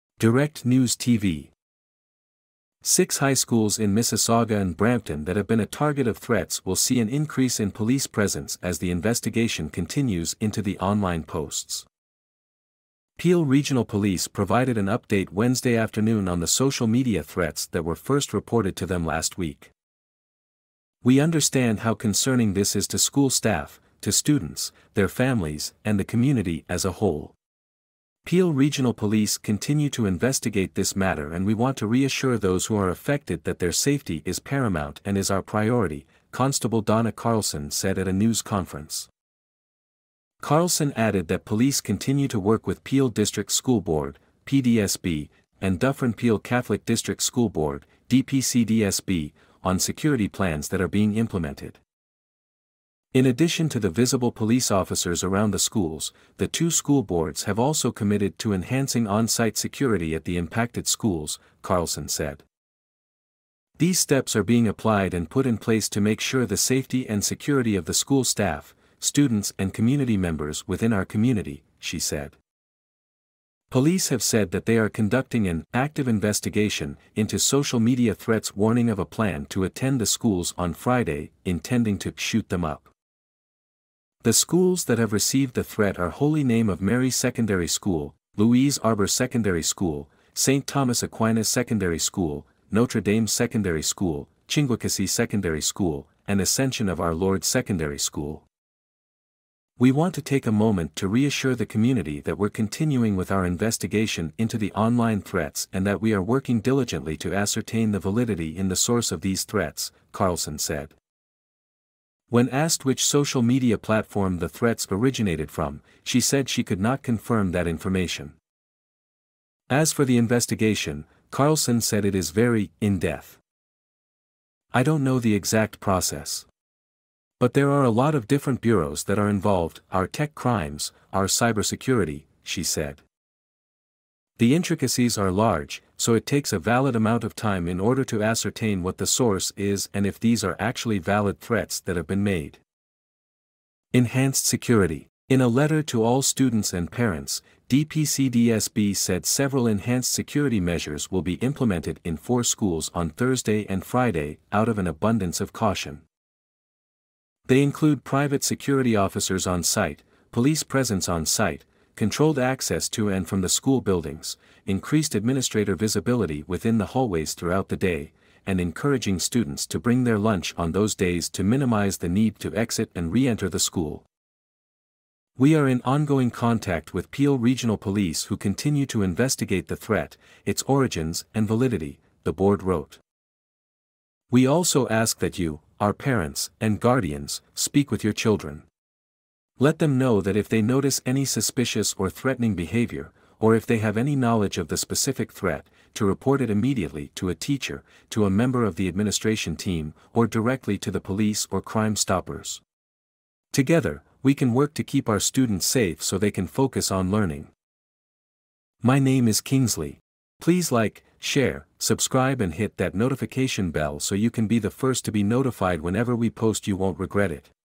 6 schools in Mississauga, Brampton target of online threats to see increased police presence
6 schools in Mississauga, Brampton target of online threats to see increased police presence